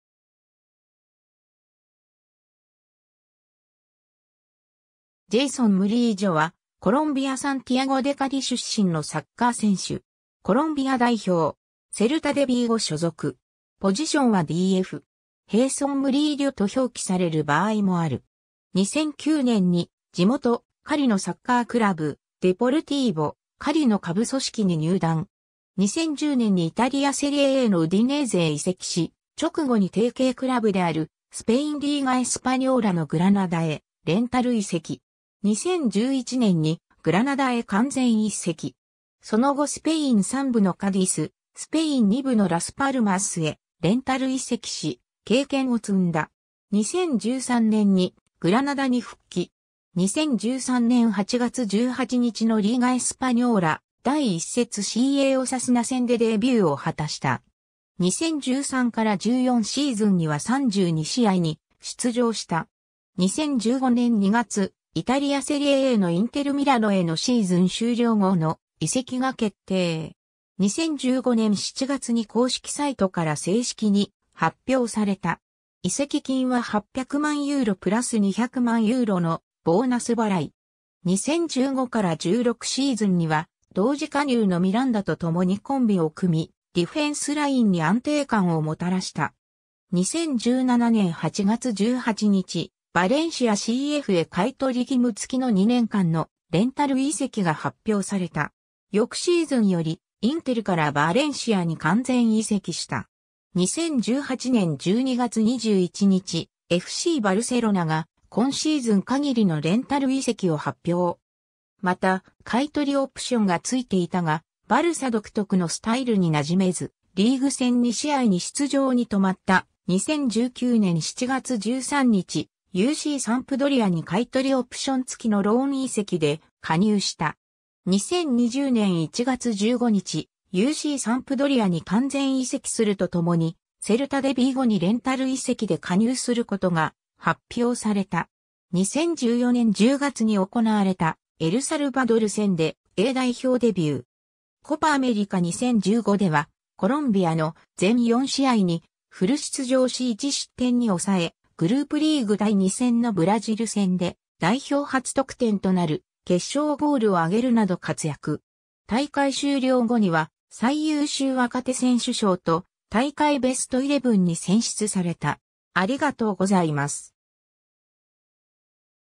ジェイソン・ムリージョ
デポルティーボ・カリの選手
クルブ・レクレアティーボ・グラナダの選手
UDラス・パルマスの選手
ジェイソン・ムリージョ（Jeison Fabián Murillo Cerón, 1992年5月27日 - ）は、コロンビア・サンティアゴ・デ・カリ出身のサッカー選手。
コロンビア代表。
セルタ・デ・ビーゴ所属。
ポジションはDF (CB)。
 ヘイソン・ムリーリョと表記される場合もある。
 2009年に地元・カリのサッカークラブ、デポルティーボ・カリの下部組織に入団。
 2010年にイタリア・セリエAのウディネーゼへ移籍し、直後に提携クラブであるスペイン・リーガ・エスパニョーラのグラナダへレンタル移籍。
 2011年にグラナダへ完全移籍。
その後スペイン3部のカディス、スペイン2部のラス・パルマスへレンタル移籍し、経験を積んだ。
 2013年にグラナダに復帰。
2013年8月18日のリーガ・エスパニョーラ第1節・CAオサスナ戦でデビューを果たした。
2013-14シーズンには32試合に出場した。
 2015年2月、イタリア・セリエAのインテル・ミラノへのシーズン終了後の移籍が決定。
移籍金は800万ユーロ+200万ユーロのボーナス払い。
2015-16シーズンには同時加入のミランダと共にコンビを組み、ディフェンスラインに安定感をもたらした。
 2017年8月18日、バレンシアCFへ買い取り義務付きの2年間のレンタル移籍が発表された。
翌シーズンより、インテルからバレンシアに完全移籍した。
 2018年12月21日、FCバルセロナが今シーズン限りのレンタル移籍を発表。
また、買い取りオプションが付いていた が、バルサ独特のスタイルに馴染めず、リーグ戦2試合に出場に留まった、 2019年7月13日、UCサンプドリアに買い取りオプション付きのローン移籍で加入した。
 2020年1月15日、UCサンプドリアに完全移籍するとともに、セルタ・デ・ビーゴにレンタル移籍で加入することが発表された。
 2014年10月に行われたエルサルバドル戦でA代表デビュー。
 コパ・アメリカ2015ではコロンビアの全4試合にフル出場し1失点に抑え、グループリーグ第2戦のブラジル戦で代表初得点となる決勝ゴールを挙げるなど活躍。
大会終了後には最優秀若手選手賞 と大会ベストイレブン に選出された。